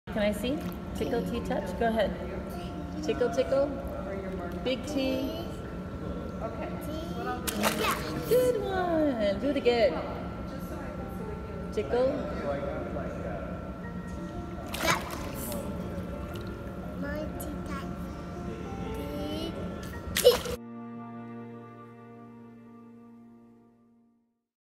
Tickle T Touch
The Tadpole Stroke: arms, but no legs

The Tadpole teaches balance and propulsion.

We swim for good times!®️